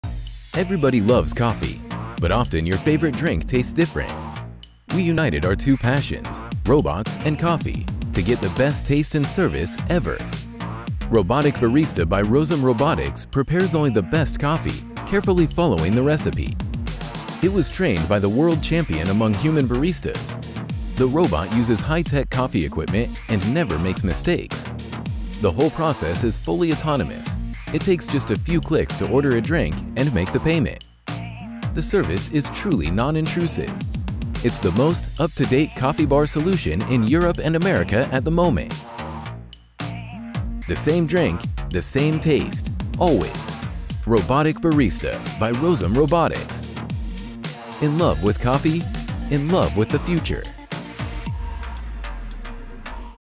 Rozum Café. Your robot barista
Rozum Café is a fully automated coffee station designed around a robot coffee maker.
A robotic barista does all the work to brew and serve great drinks. The service is quick, smart, and blends seamlessly into the background to let you enjoy the coffee experience.
Rozum Café is available to buy on the white label terms.